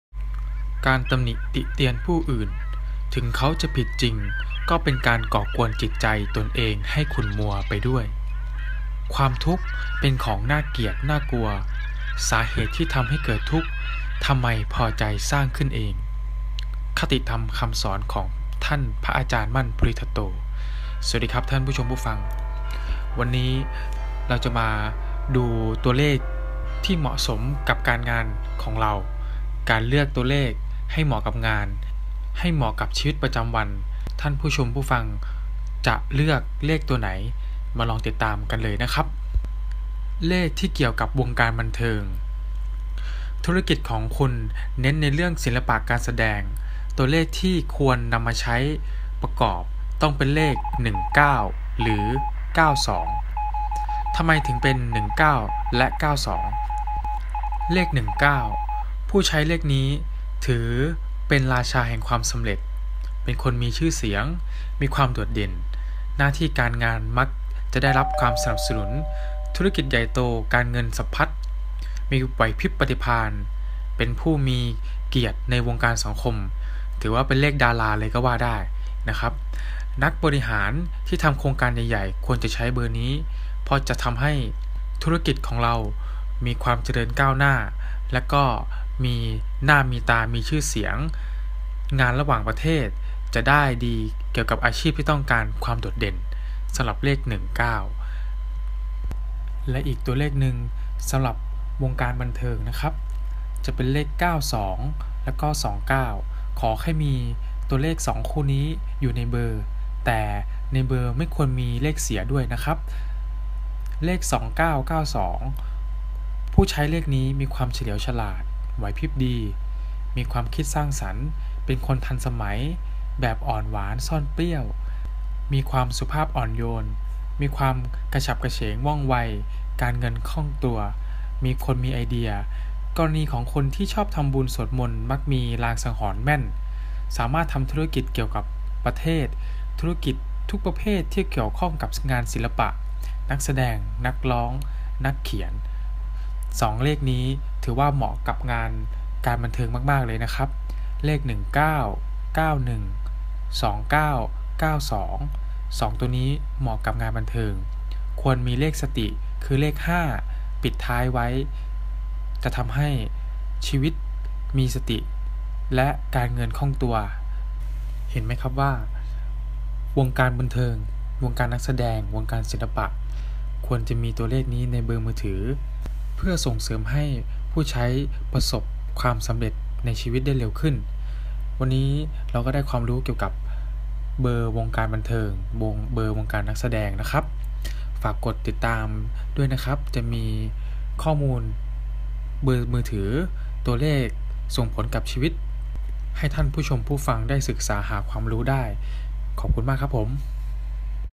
เบอร์มือถือ สำหรับนักแสดง วงการบันเทิง EP.3
เลือกเบอร์ให้เหมือนกับงาน
ตอนเบอร์เด่นดัง ต้องการความโดนเด่น งานศิลปะ การแสดง
สนใจวิเคราะห์เบอร์ฟรี